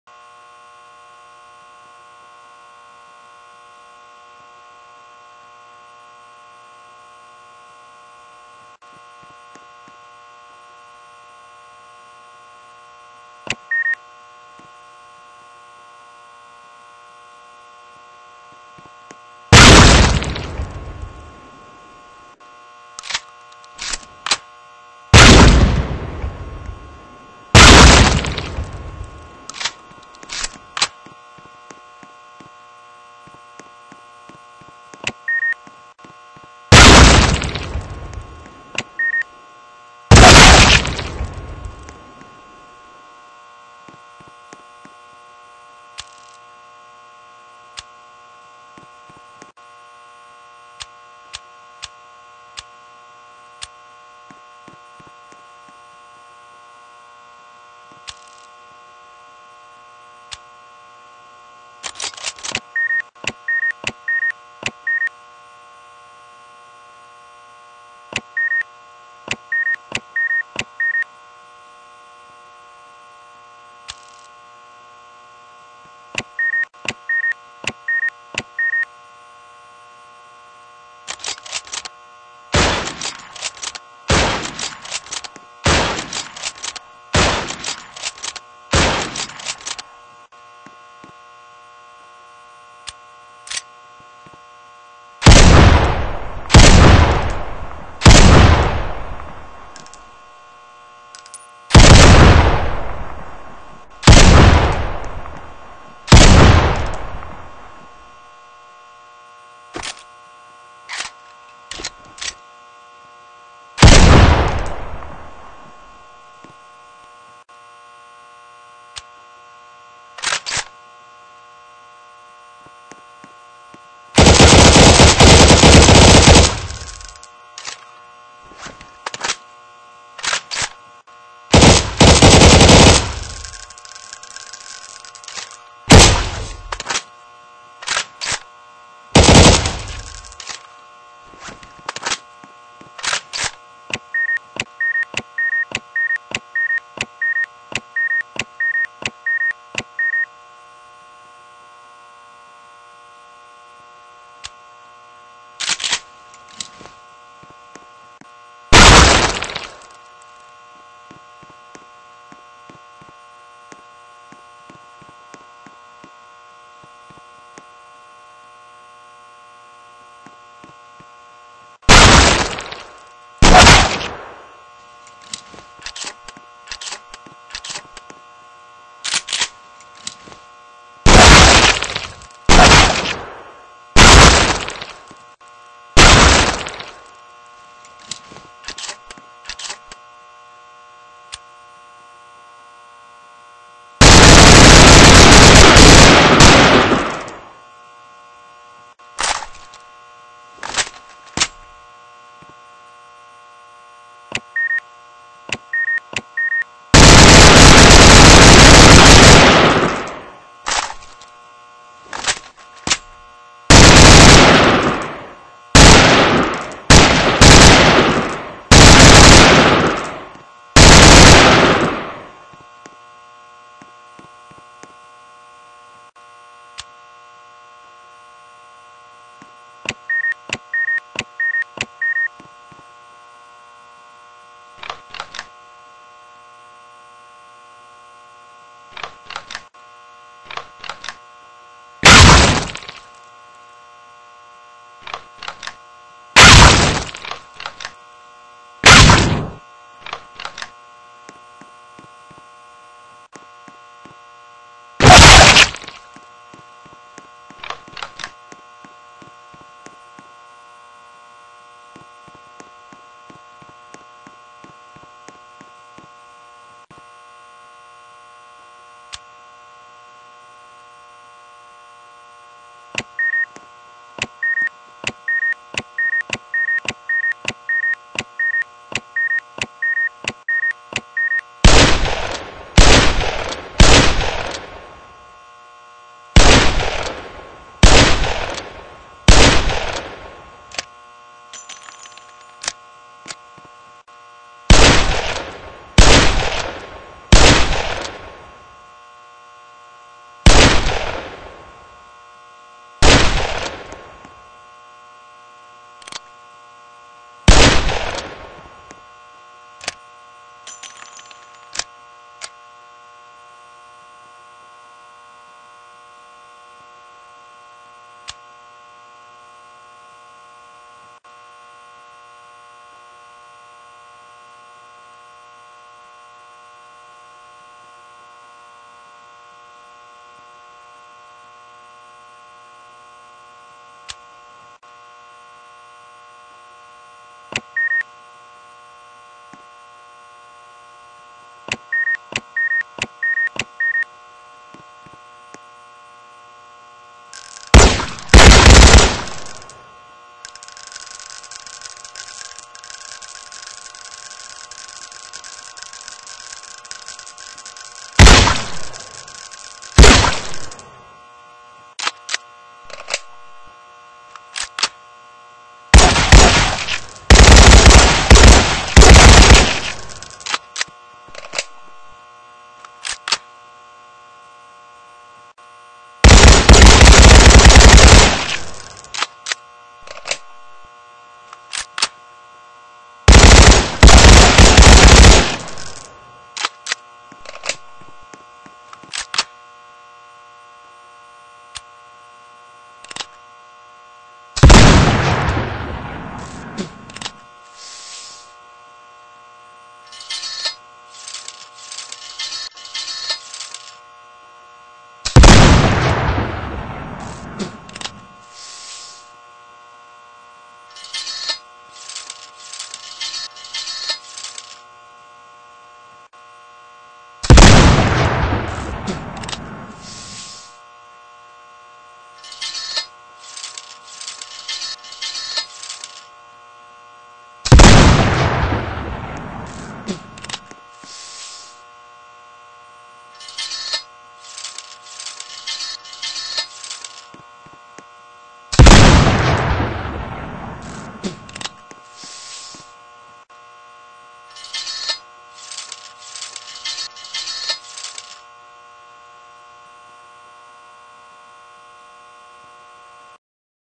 Glory kill testing : Je tue des zombies avec des armes!!
Attention dans cette vidéo y'a du sang et gore.